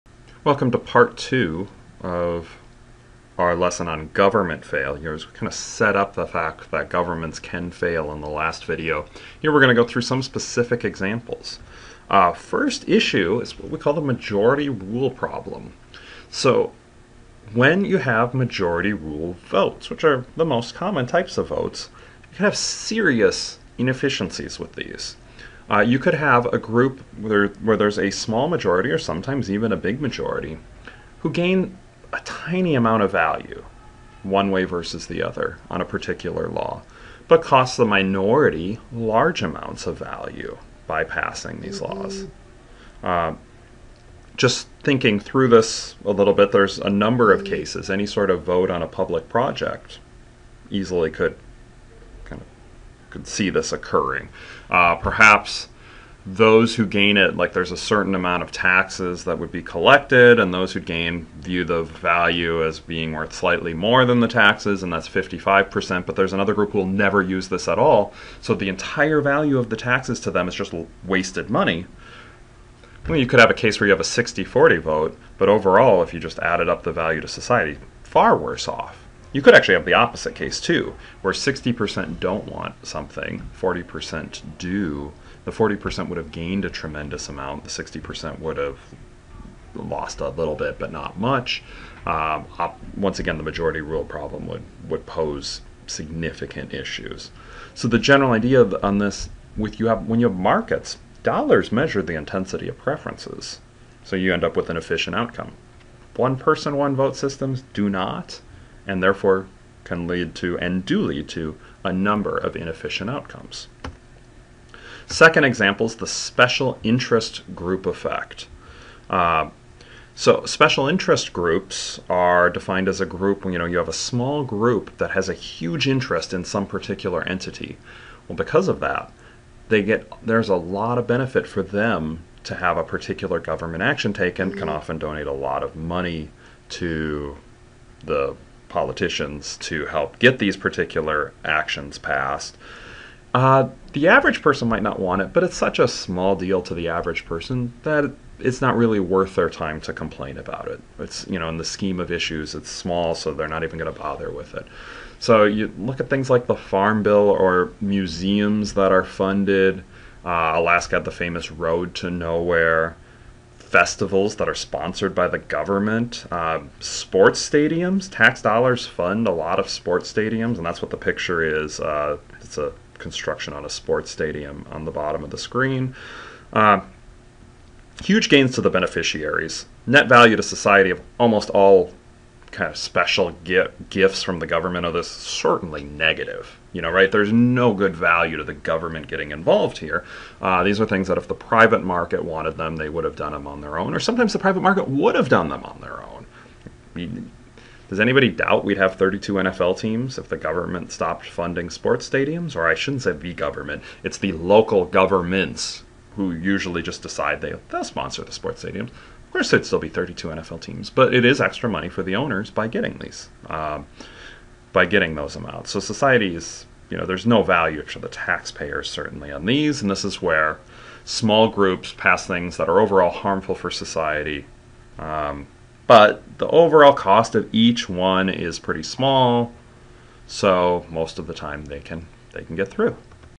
Principles of Microeconomics- Public Choice Theory/Government failures (part 2)
Dr. Rousu teaches undergraduate Principles of Microeconomics. In this lesson, he covers the majority rule problem and special-interest group effect.